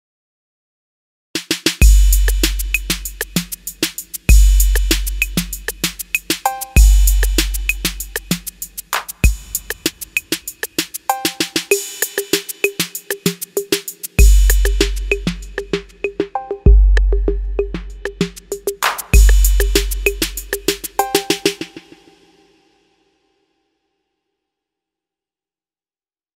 BPB Cassette 808 (Free 808 Drum Sample Pack)
Free 808 sample pack by BPB, based on Gregor Zemljic's excellent Roland TR-808 drum machine recordings. The pack contains 87 high quality 808 drum samples in 24-bit WAV format. We have also included a custom instrument patch for NI Kontakt 5. Enjoy!